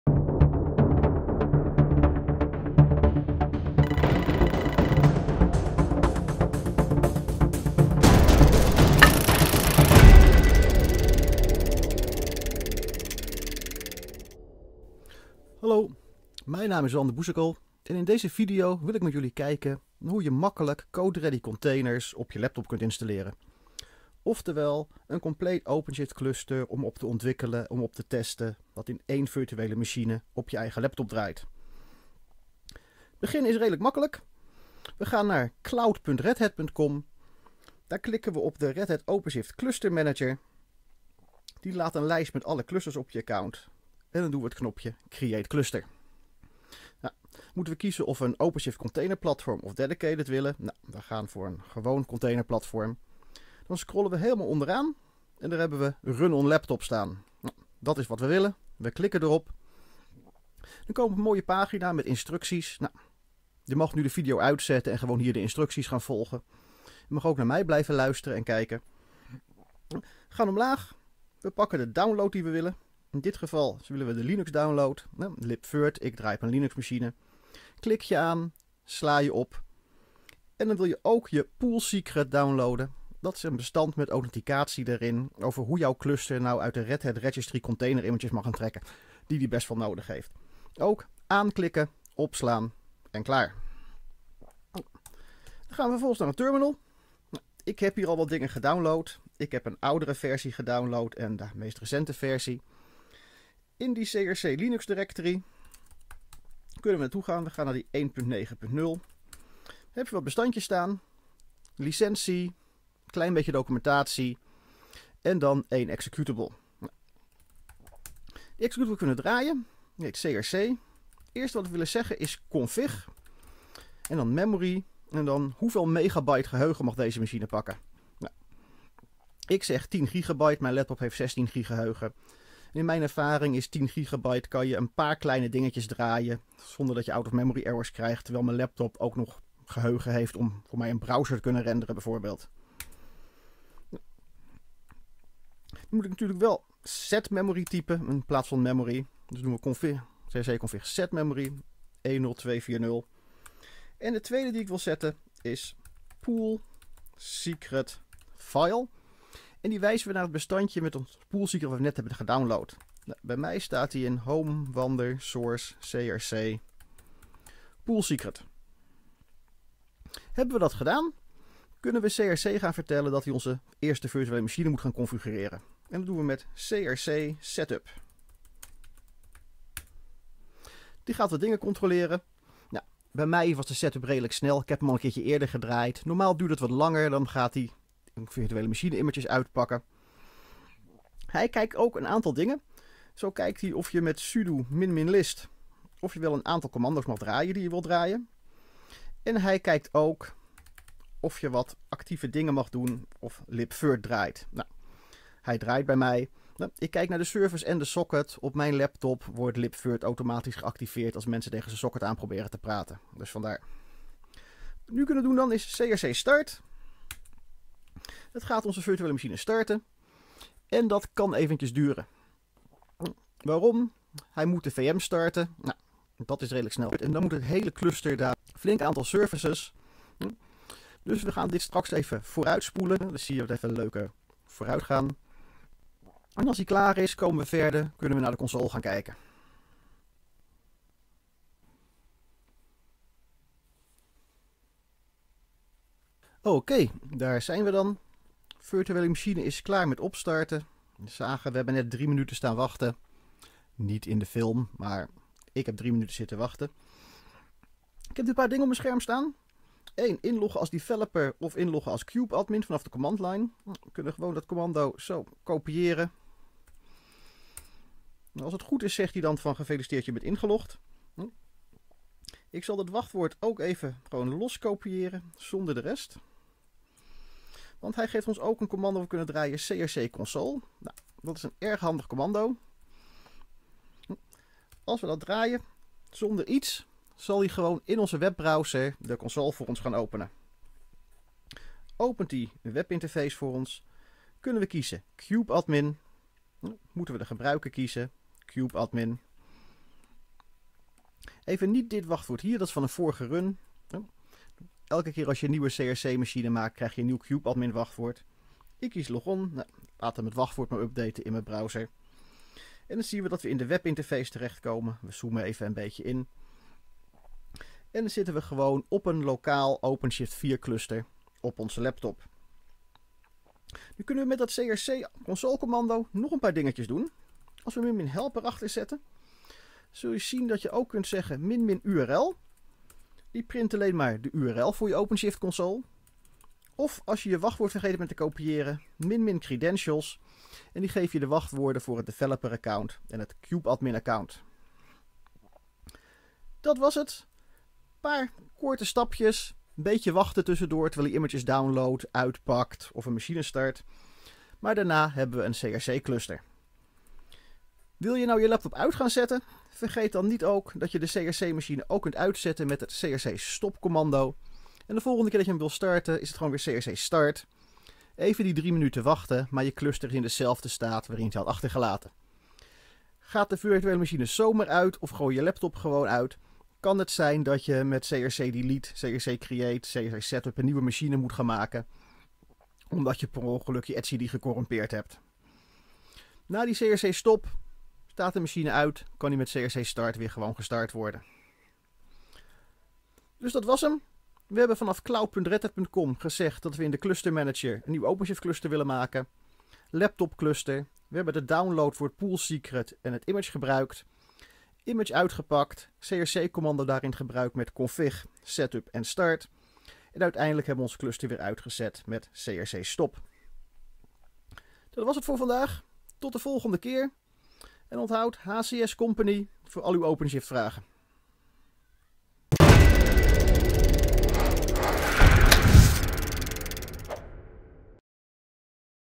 OpenShift: CodeReady Containers Installeren - HCS Company Inside
Tutorial OpenShift: CodeReady Containers installeren. In deze video bekijken we hoe je gemakkelijk een volledig OpenShift cluster in één VM op je laptop/desktop kunt installeren. Ideaal voor snelle testen en ontwikkelen onderweg.